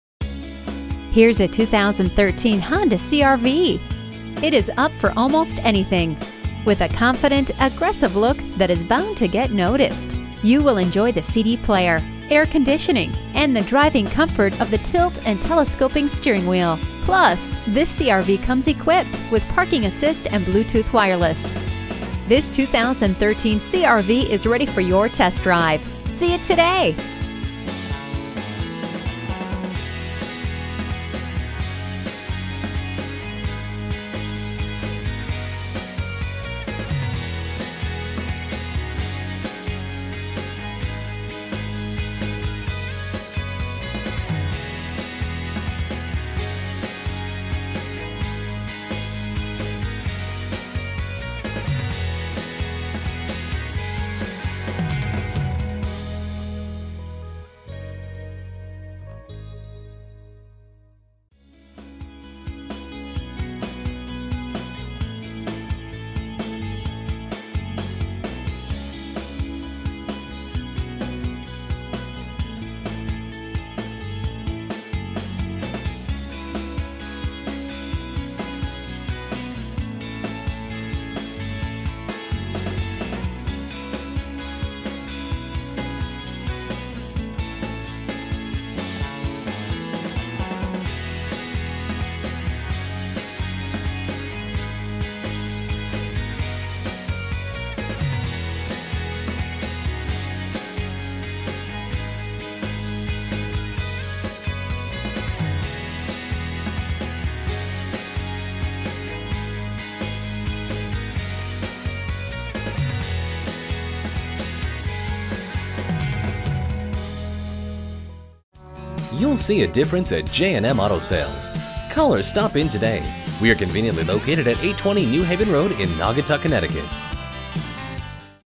2013 Honda Cr-v Awd Naugatuck CT Hartford, CT 137111T - SOLD
Year: 2013
Make: Honda
Model: Cr-v Awd
Trim: 5d Wagon EX
Engine: -1 Cylinders
Color: Tan
Mileage: 45665
Stock #: 137111T
VIN: 5J6RM4H58DL027111
A Desirable, One-Owner CR-V EX with an Accident-Free History Report and an Included Warranty! This All Wheel Drive Beauty is Loaded with Grey Interior, a Power Sunroof, Alloy Wheels, Climate Control, Power Seats, Power Windows, Power Door Locks, Power Mirrors, an Adjustable Steering Column, Cruise Control, a 2.4L VTEC 4-Cylinder Engine, an Automatic Transmission and More! A High Quality Find at J&M!.

Address:
820 New Haven Rd.